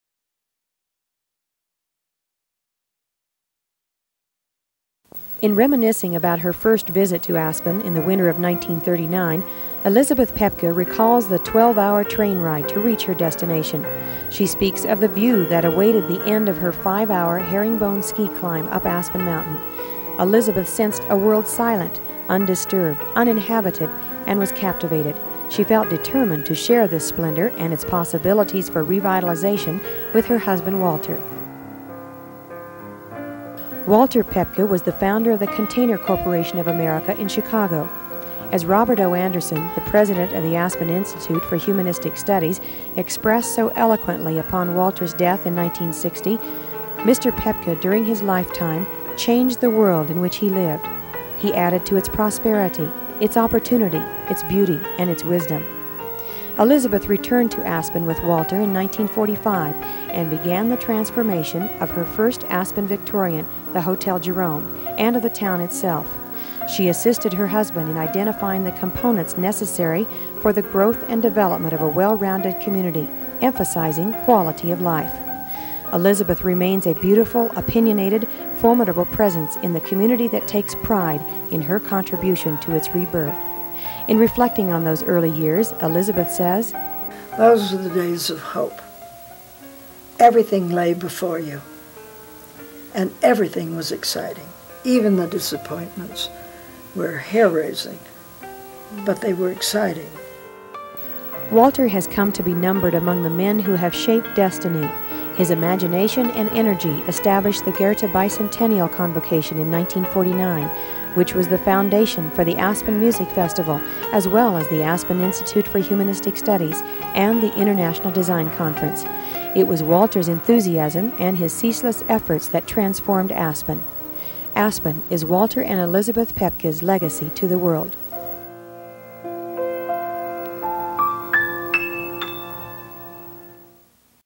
Elizabeth & Walter Paepcke 1987 Inductees into the Aspen Hall of Fame
Elizabeth and Walter Paepcke were inducted into the Aspen Hall of Fame in 1987.